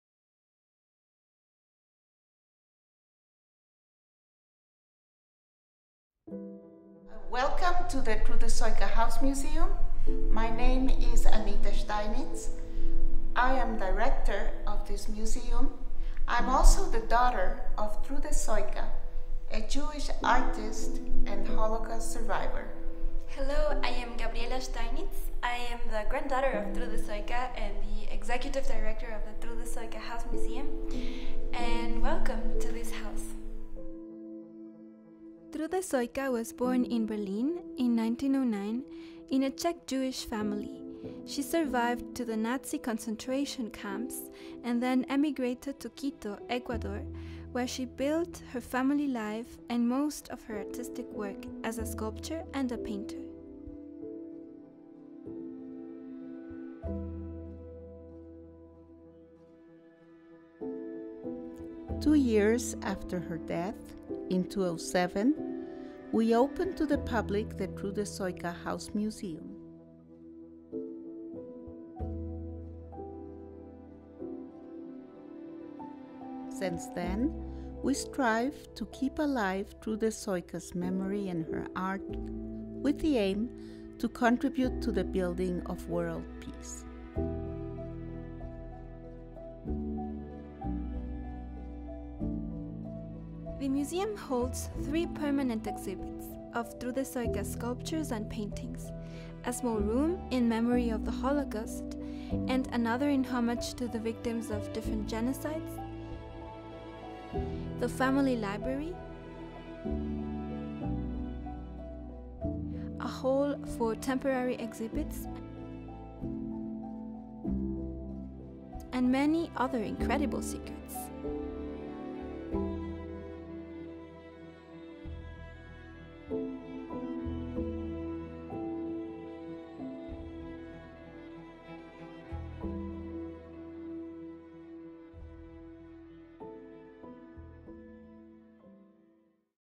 The Trude Sojka House Museum
Did you know there is a museum in Quito, Ecuador, dedicated to a Holocaust survivor and artist, Trude Sojka? Meet Gabriela and Anita Steinitz, descendants of Trude Sojka, who will guide you a little bit through the Trude Sojka House Museum.